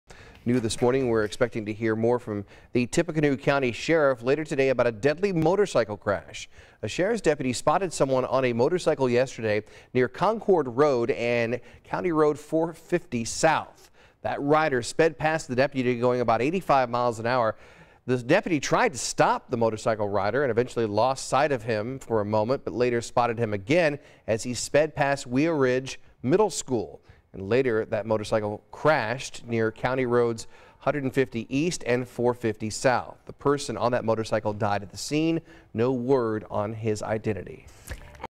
1 dead after motorcycle crash in Tippecanoe County
It happened Tuesday at a roundabout at County Roads 150 East and 430 South. Police said the rider sped past a deputy going about 85 miles per hour.